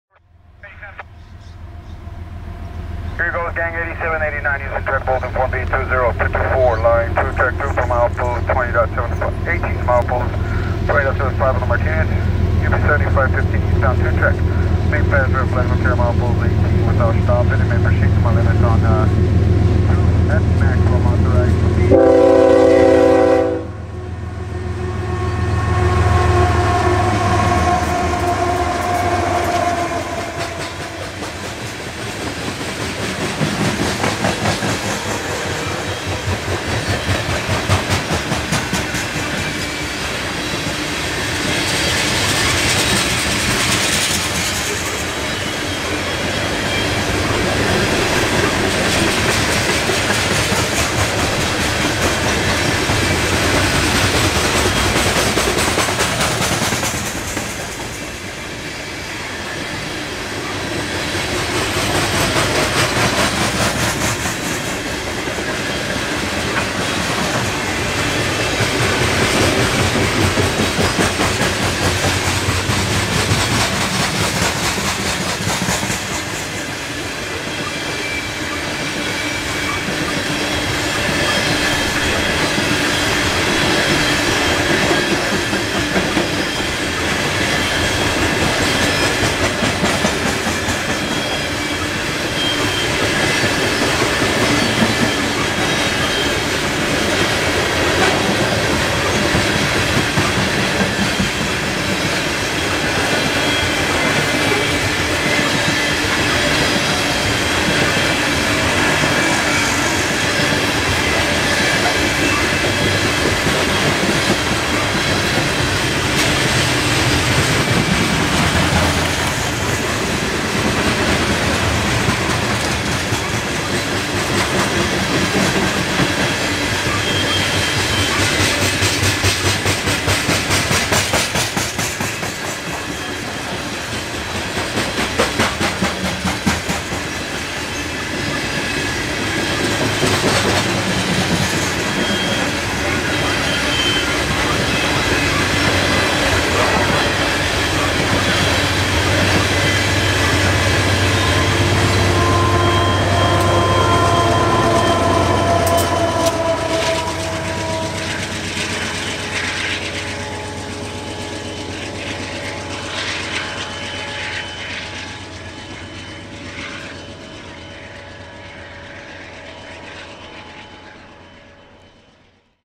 UP 5484 Coal Train Through Hercules 05sep17
As the UP 5484 heads up the coal train, you can hear a track warrant being read to the UP 7515 approaching from the other direction (ebound), heading up a bare table train. Video to follow.